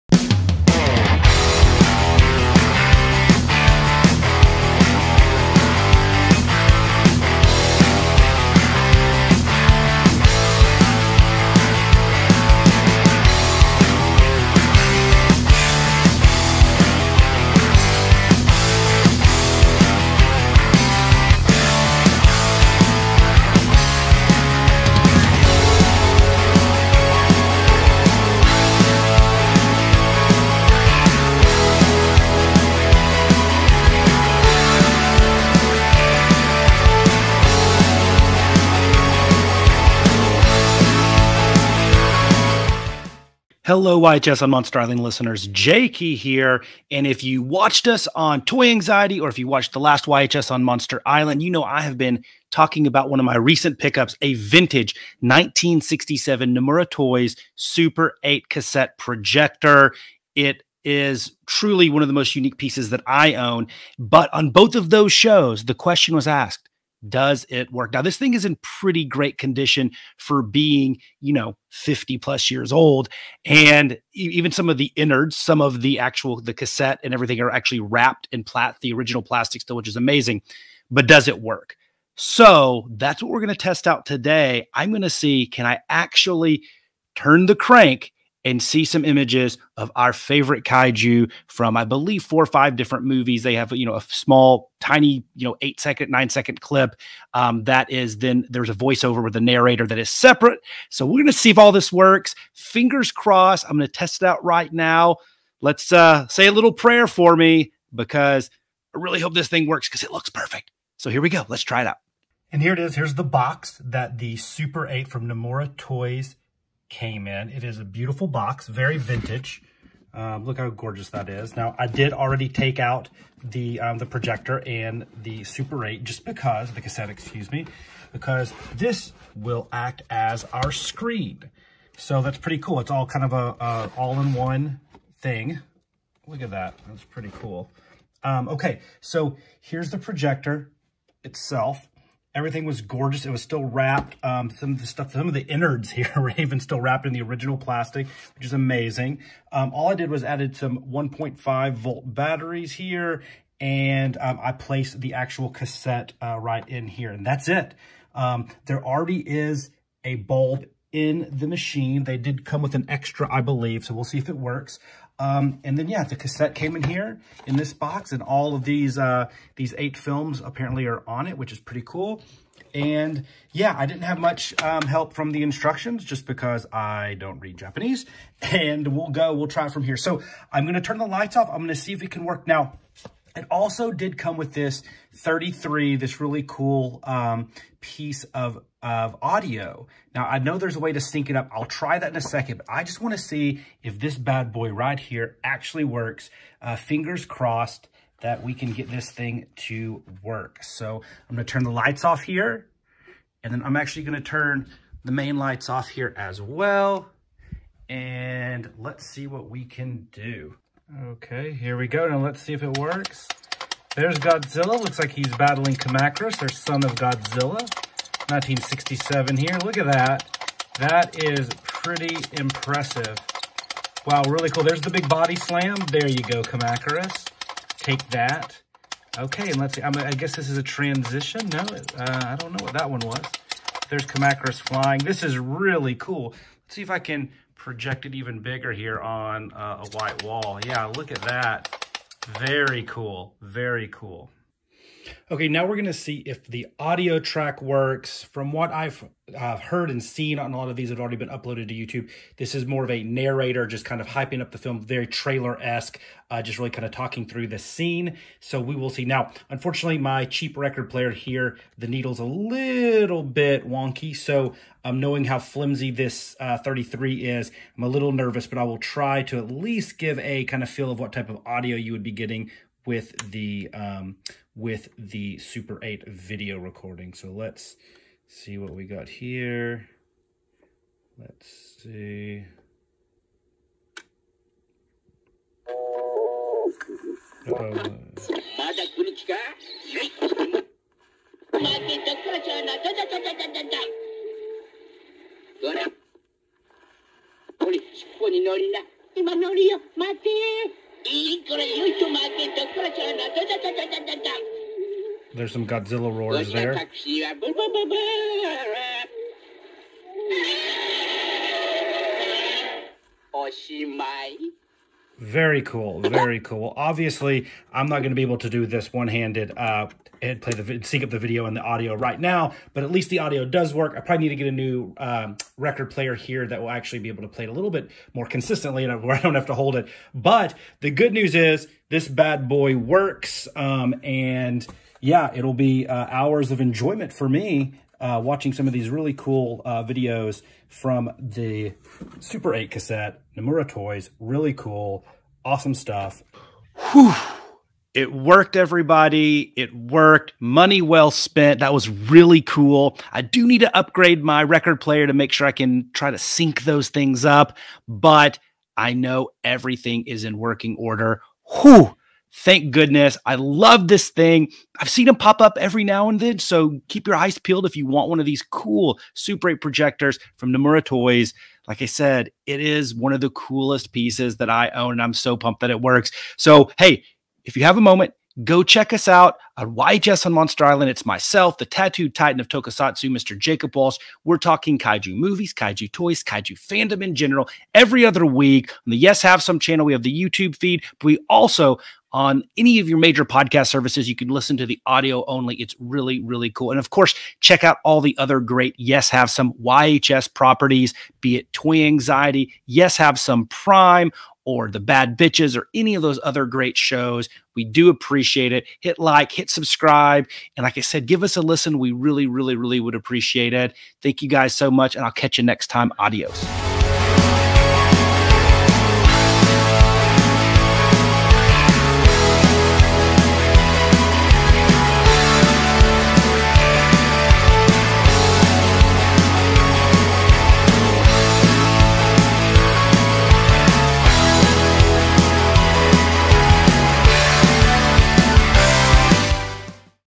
SOFUBI STRESS - Cassette 8 Movie Show Projector (Nomura Toys) Godzilla! DOES IT WORK? Kaiju Toys
**ORIGINALLY AIRED ON NOVEMBER 17, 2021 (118 views)**

Join YHS on Monster Island's Jay Key in this new installment of SOFUBI STRESS! Some people have toy anxiety...but kaiju collectors have sofubi stress!

In this episode, Jay attempts to get his vintage 1967 Nomura Toys Cassette 8 Movie Show Projector to work...but does it? This projector features 5 Godzilla film excerpts...including clips from Son of Godzilla, Godzilla vs. the Sea Monsters, Ghidorah, the Three-Headed Monster, and Mothra vs. Godzilla!